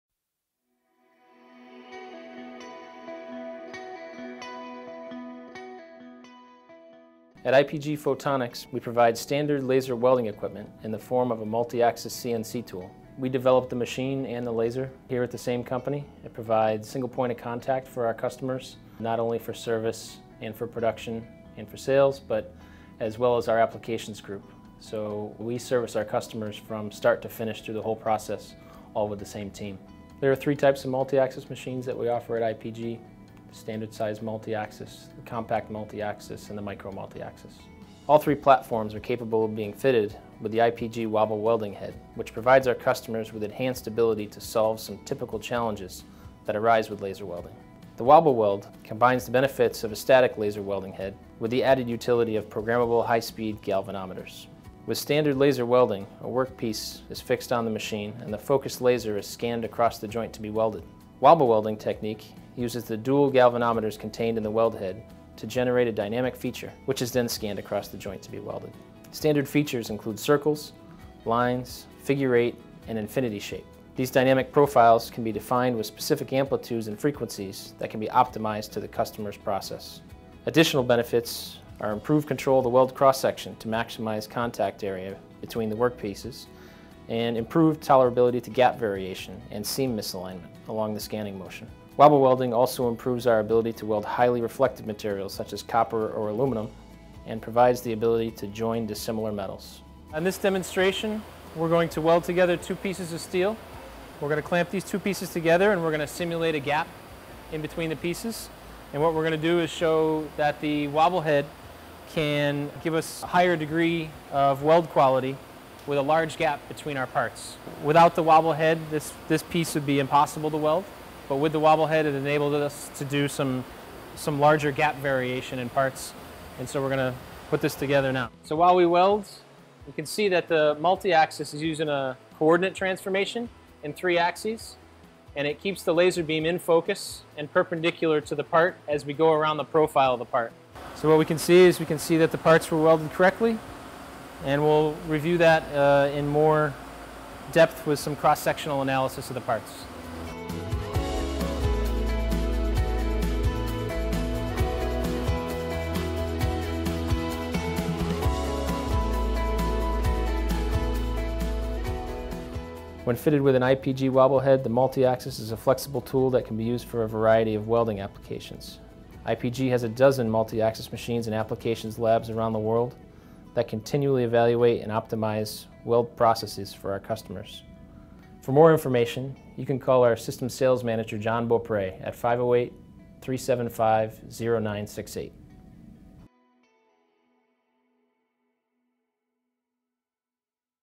What is Wobble Welding?
In this video, discover how IPG’s wobble welding technology delivers superior weld quality and a better visual finish, even when parts have less-than-perfect fit-up. By widening the area covered by the focused laser beam, wobble welding dramatically expands the process parameter window and reduces the impact of process variables on the final seam.